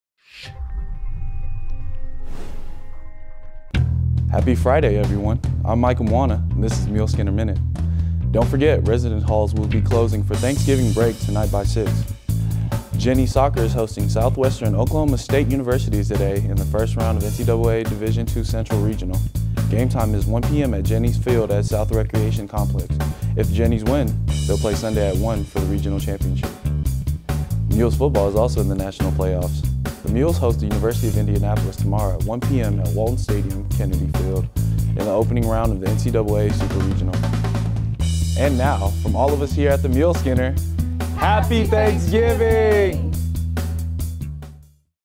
Muleskinner Minute - November 22, 2019
Michael Moana and the Muleskinner staff share about some upcoming events and holiday greetings in this episode of Muleskinner Minute.